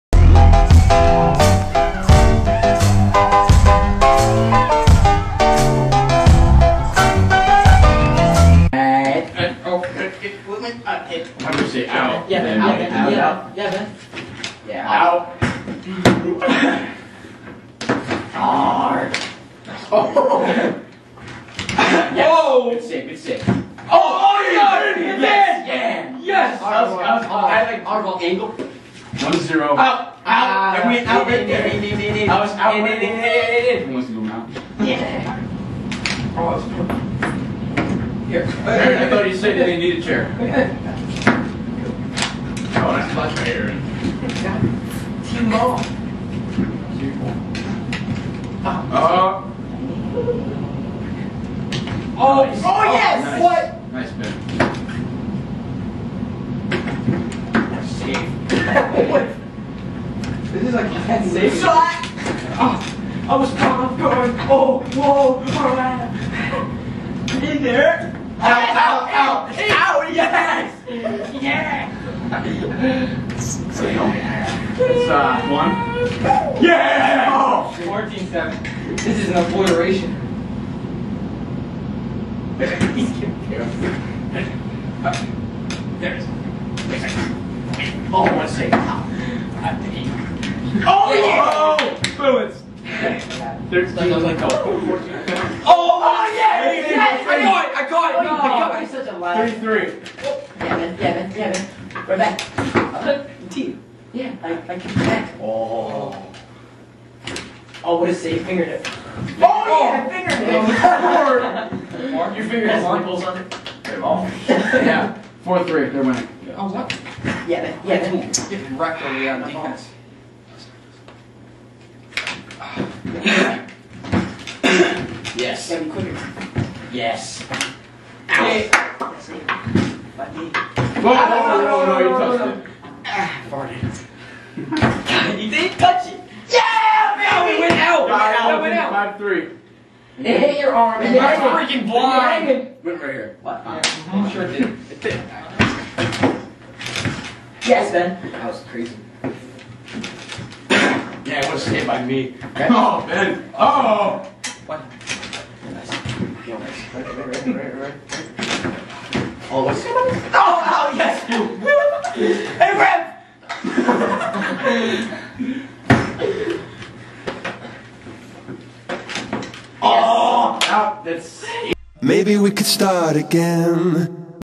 Table Hockey
On a Stormy Saturday in early October, and exhibition match of the STHLA took place between Pommer+Fontana & Fahrenz+Constantino...

Here are some of the highlights...